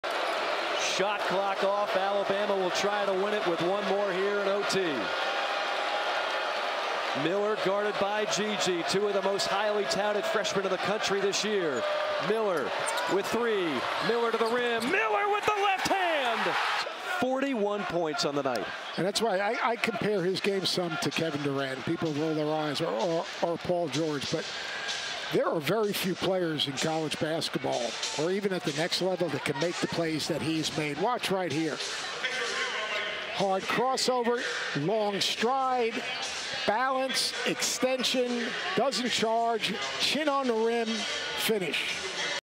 Brandon Miller scores the game-winner and a career-high 41 points in road win vs the Gamecocks 👀
Brandon Miller weaves his way to the basket for the game-winner and a career-high 41 points in the 78-76 OT thriller on the road vs the Gamecocks 👀